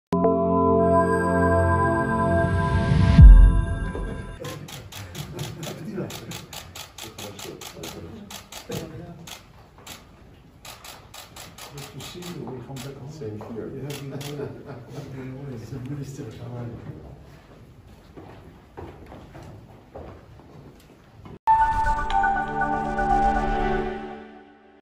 لعمامرة يتحادث بموسكو مع سيرغي لافروف
الجزائر (وأج) - عقد وزير الشؤون الخارجية والجالية الوطنية بالخارج, رمطان لعمامرة, بالعاصمة الروسية موسكو, جلسة عمل مع نظيره الروسي, سيرغي لافروف,  حول محاور التعاون بين البلدين وآفاق تعزيزه.